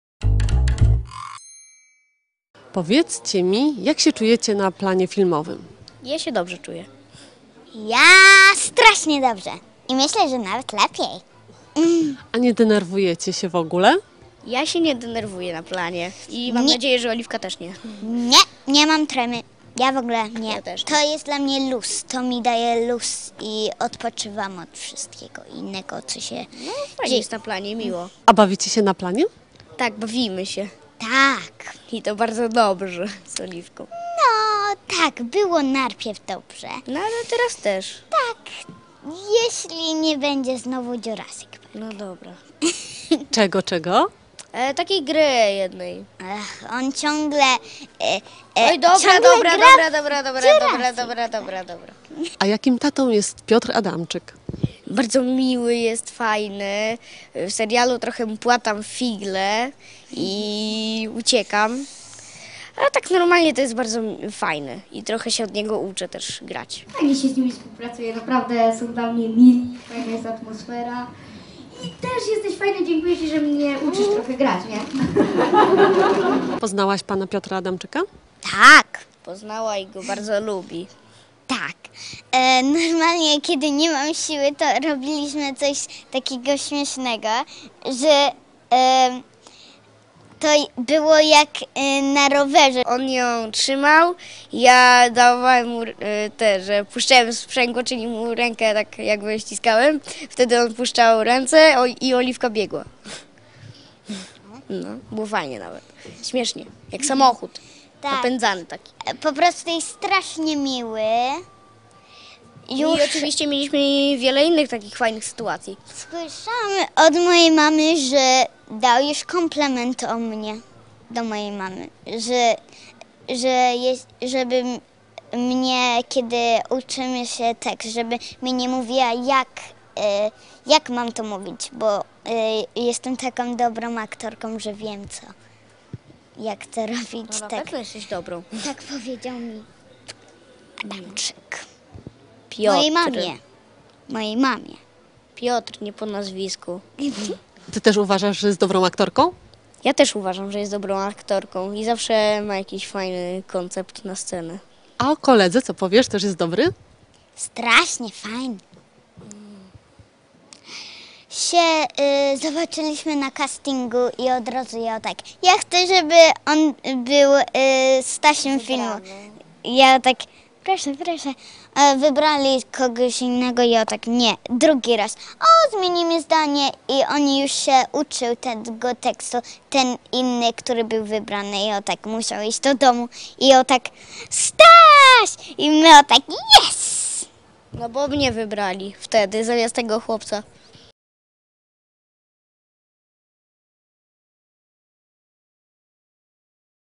Dziecięce gwiazdy serialu to tytułowa "Sama słodycz"! "Nie mam tremy, to mi daje luz!" [Plotek]
Olaf Marchwicki i Olivia Kupczyński to dziecięce gwiazdy nowego serialu 'Sama słodycz'. Już wiemy, skąd tytuł produkcji. Tylko posłuchajcie tego, co mówią.